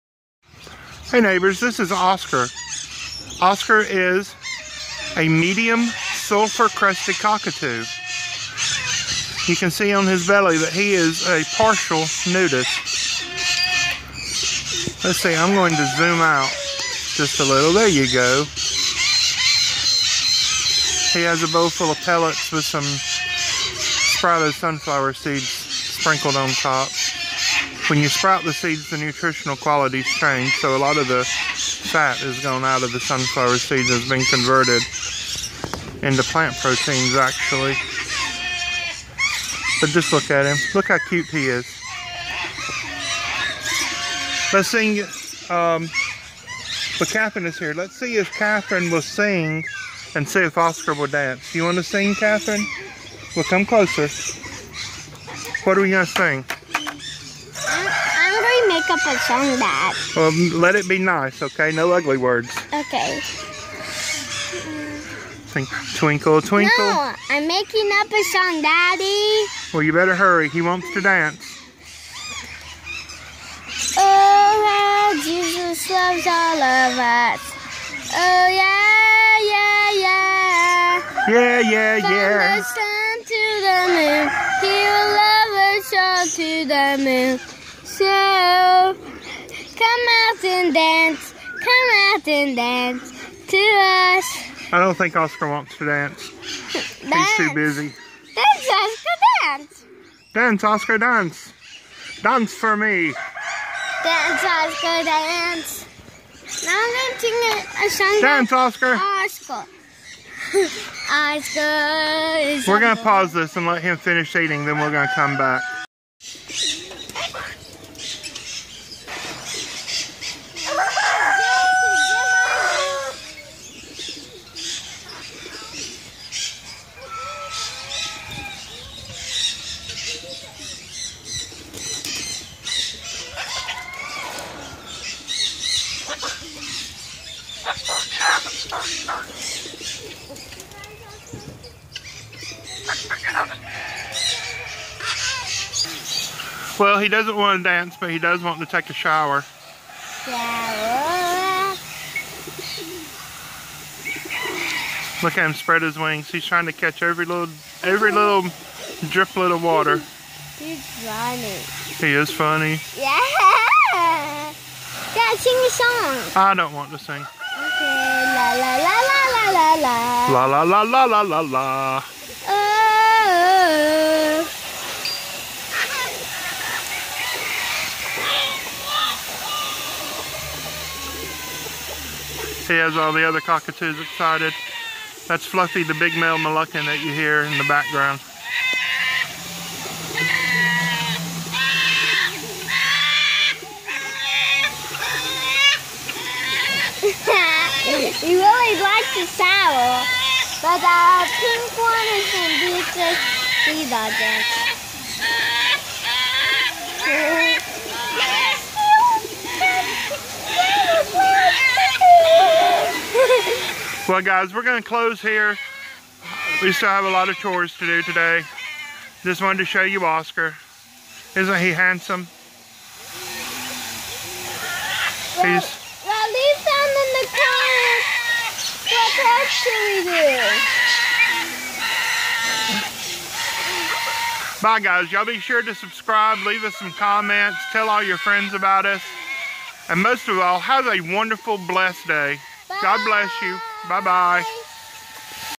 TALKING, DANCING COCKATOO - Meet Oscar
He talk.  He dances.  He sings.   He's a lot of fun.

Oscar is a subspecies of the Greater Sulfur Crested Cockatoo known as The Eleonora Cockatoo.   Isn't he wonderful?

He' is in need of a 5-10 year old, confident female to be his mate.  If you know of one, please let us know.

There are two species of sulfur crested cockatoos.  The Greater and the Lesser.  The Greater has four subspecies and the Lesser has two subspecies.

The Greater Subspecies are

Cacatua galerita galerita  - The Galerita Cockatoo
C. g. triton - The Triton Cockatoo
C. g. eleonora - The Eleonora Cockatoo
C. g. fitzroyi - The Fitzroy Cockatoo

The nominate subspecies, The Galerita, is considered an agricultural pest species in its native Australia.  :(

There are four subspecies of the Lesser Sulfur Crested Cockatoo, also known as the Yellow Crested Cockatoo.  These are smaller than the others.

Cacatua sulphurea sulphurea - The Lesser Sulfur Crested
Cacatua sulphurea abbotti - The Abbott's Lesser Sulphur Crested
Cacatua sulphurea citrinocristata - The Citron Crested Cockatoo
Cacatua sulphurea parvula - The Timor Sulfur Crested Cockatoo

The Lesser Sulfur Crested Cockatoos are all listed as Endangered.

Here's a great book that teaches about Cockatoos

And you can go ahead and order a 2018 calendar:

And I know you've always wanted a cockatoo coloring book: